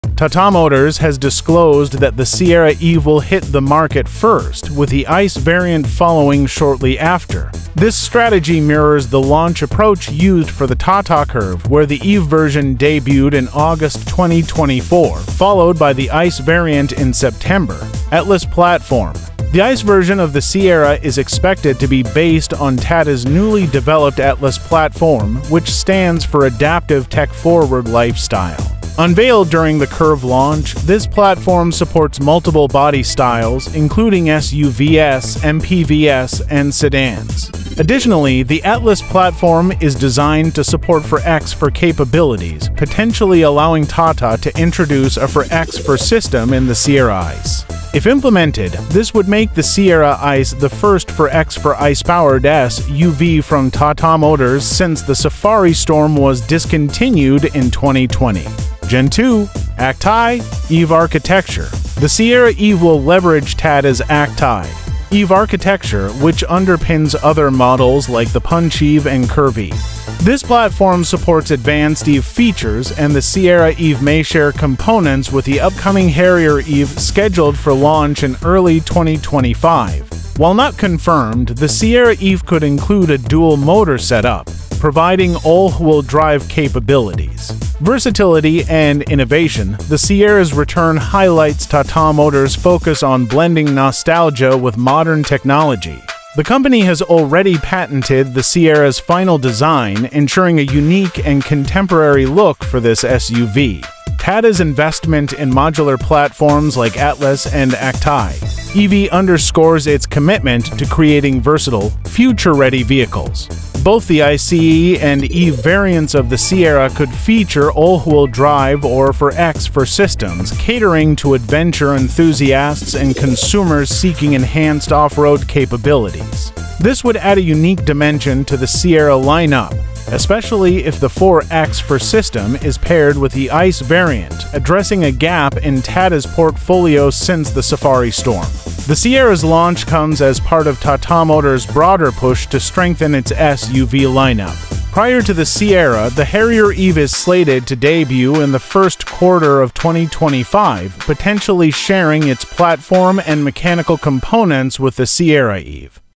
Tata Sierra EV Launch: First Electric, ICE Variant Soon After!
Tata Motors is all set to make waves in the SUV market with the return of the iconic Tata Sierra, but this time, it's going electric! Tata has confirmed that the Sierra EV will hit the market first, followed closely by the ICE (Internal Combustion Engine) variant. This strategy follows the successful launch approach of the Tata Curvv, where the EV version debuted before the ICE version in 2024.

ATLAS Platform for ICE Variant
The Sierra ICE will be based on Tata’s new ATLAS platform, designed to support various body styles, including SUVs, MPVs, and sedans. The platform also supports 4X4 capabilities, which might mean the return of a 4X4 ICE-powered SUV from Tata. If this happens, the Sierra ICE could be the first Tata SUV since the Safari Storme to offer a 4X4 drivetrain. It would be a game-changer for those looking for a rugged, off-road capable vehicle in the mid-size SUV segment.

Gen2 Acti.EV Architecture for the Sierra EV
The Sierra EV will use Tata's Gen2 Acti.EV architecture, a platform that powers other upcoming electric vehicles like the Punch EV and Curvv EV. With advanced EV features, the Sierra EV might share components with the upcoming Harrier EV, set to launch in early 2025. Although not confirmed, there are talks of a dual-motor setup, potentially offering all-wheel drive (AWD) for a more dynamic driving experience.

Versatility and Innovation
Tata Motors is blending nostalgia with cutting-edge technology in the Sierra’s design. The final design has already been patented, ensuring a unique and modern look that will appeal to both legacy and new customers. Tata's investment in modular platforms like ATLAS and Acti.EV underscores its commitment to creating vehicles that are both versatile and future-ready.

All-Wheel Drive and 4X4 Options
Both the Sierra EV and Sierra ICE might feature AWD or 4X4 systems, providing enhanced off-road capabilities for adventure lovers. The 4X4 option in the ICE variant would be particularly exciting, filling a gap left since the discontinuation of the Safari Storme.

Tata Motors’ Roadmap for 2025
Tata Motors’ aggressive push to strengthen its SUV lineup continues with the Sierra’s launch, set against the backdrop of the Harrier EV debuting in Q1 2025. Both vehicles will likely share platforms and mechanical components, making them the perfect pair for Tata’s growing electric and hybrid portfolio.

Will you choose the electric version or go for the 4X4 ICE variant for some off-road adventures?